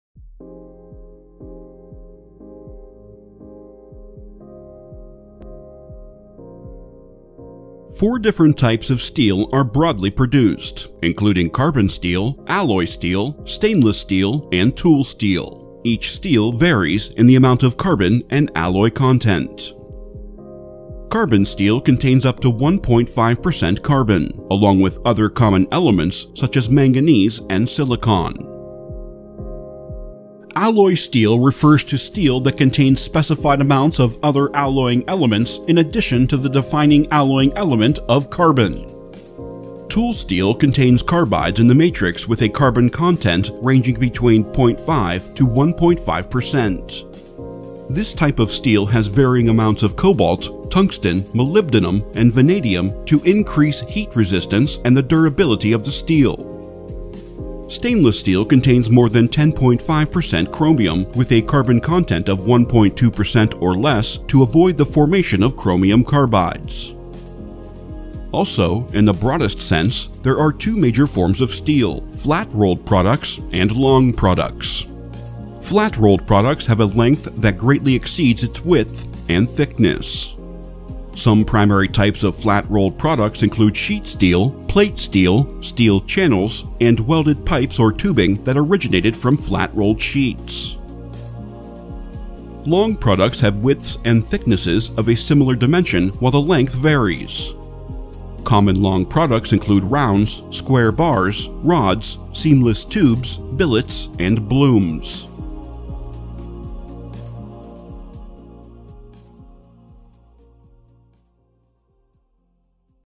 What Are Types and Forms of Steel? || THORS Steel Basics Course Preview
What are 4 types and 2 forms of steel? Find out in this preview for the Steel Basics course from THORS eLearning Solutions.

Steel Basics | Course Learning Objectives
1. Recall the primary types of steel.
2. Recognize the basic forms in which steel is manufactured.
3. Retain the processing steps required in the manufacturing of steel products.
4. Differentiate between the important mechanical property tests applied to steel.
5. Determine the difference in the primary methods used for assigning steel grades.